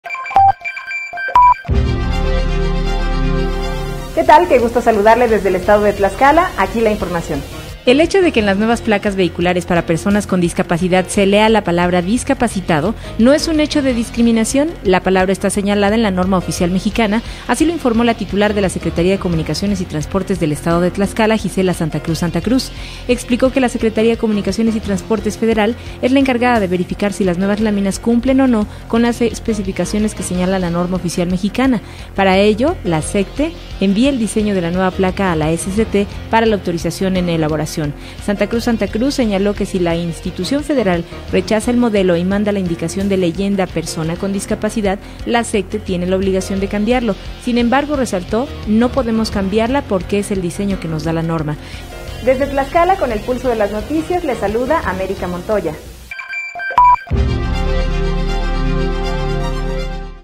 LA LEYENDA DISCAPACITADOS EN LAS PLACAS, DENTRO DE LA NORMA.mov
ASEGURA LA TITULAR DE LA SECRETARÍA DE COMUNICACIONES Y TRANSPORTES DEL ESTADO QUE LA LEYENDA "DISCAPACITADOS" DENTRO DE LAS PLACAS ESTÁ MARCADO EN LA NORMA OFICIAL MEXICANA.